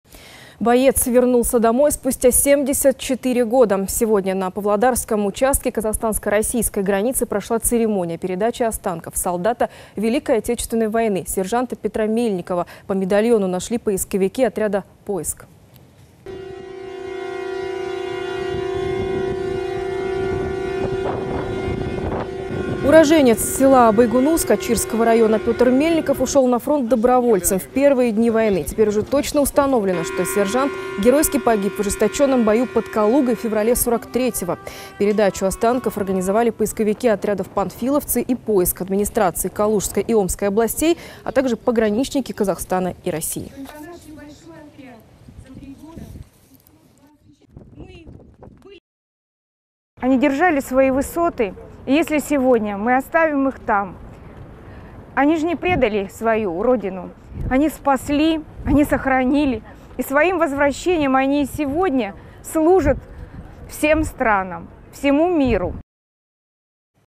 Останки бойца ВОВ привезли в Казахстан
Боец вернулся домой спустя 74 года.

Сегодня на Павлодарском участке казахстанско-российской границы прошла церемония передачи останков солдата Великой Отечественной войны. Сержанта Петра Мельникова по медальону нашли поисковики отряда «Поиск».

Уроженец села Байгунус Качирского района Петр Мельников ушел на фронт добровольцем в первые дни войны. Теперь уже точно установлено, что сержант геройски погиб в ожесточенном бою под Калугой в феврале 1943 года.

Передачу останков организовали поисковики отрядов «Панфиловцы» и «Поиск», администрации Калужской и Омской областей, а также пограничники Казахстана и России.

Галина Кудря, поисковик (Россия):

- Они держали свои высоты,  они же не предали свою Родину. Они спасли, они сохранили, и сегодня своим возвращением служат всем странам, всему миру.